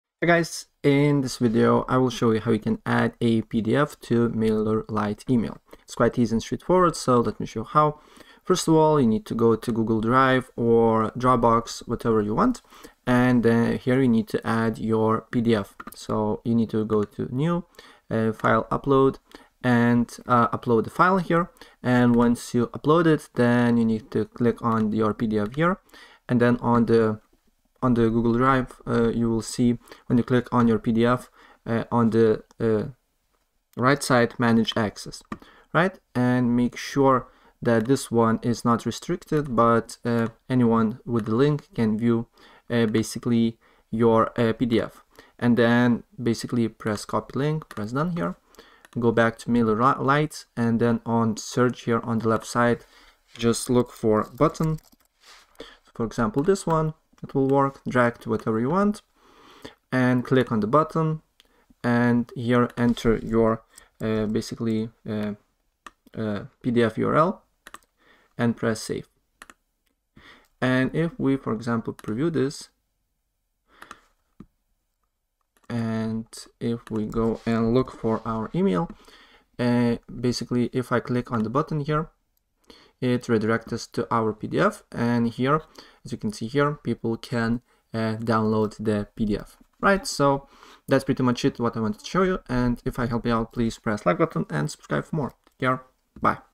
How To Attach PDF To MailerLite Email | Add PDF File
In this video I will show you How To Attach PDF To MailerLite Email | Add PDF File It's really easy and it will take you less than a minute to do so!